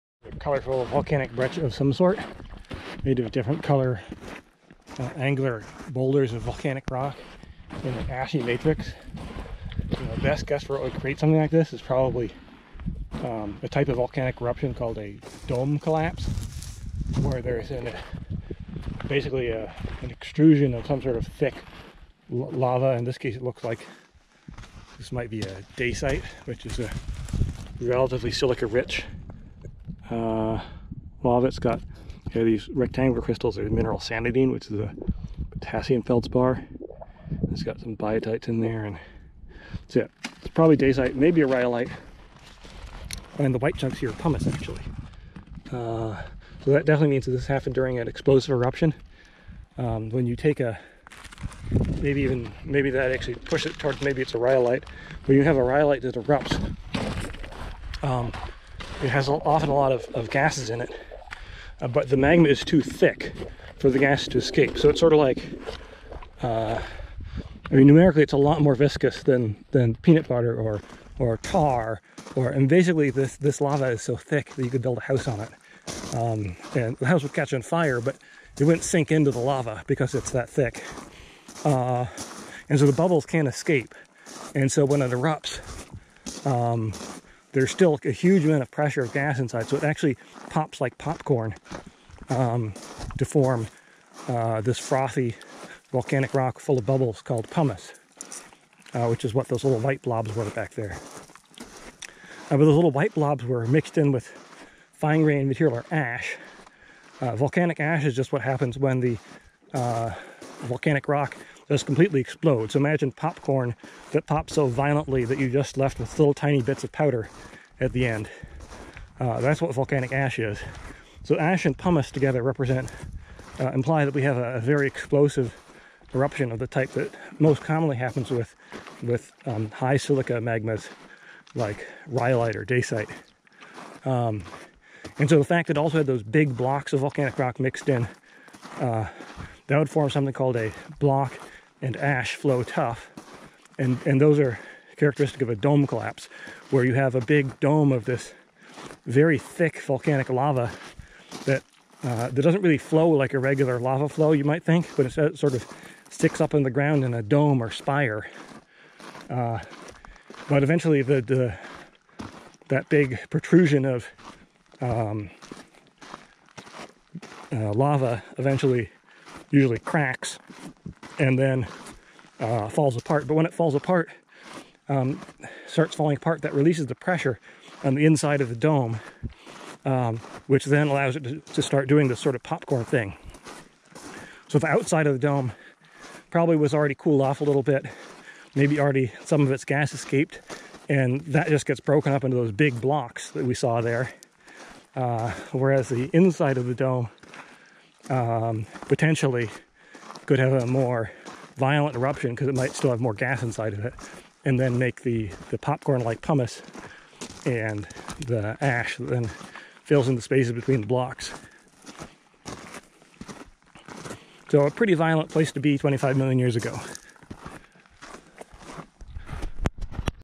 Volcanic dome collapse: Mojave Trek Daily 2.9
We discuss the origin of a colorful volcanic breccia, or block-and-ash tuff